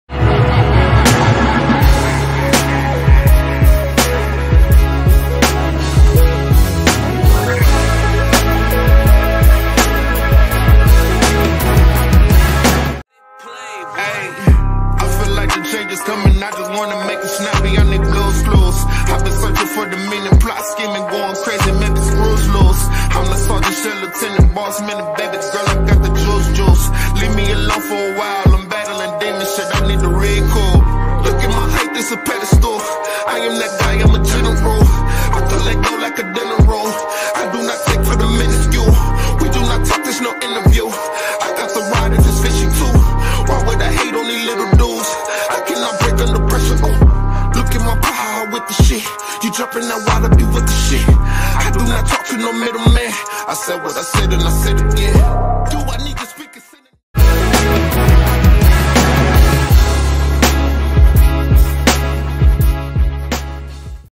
Walt Trab (@Trab4short ) Performs at Coast 2 Coast LIVE | Milwaukee 11/12/19 - 4th Place
Artist
Walt Trab - Walt Trab is an up and coming artist originally from Milwaukee, WI. He has been creating music seriously for 6 plus years now.

Walt Trab ...

Tuesday, November 12, 2019 9:00 PM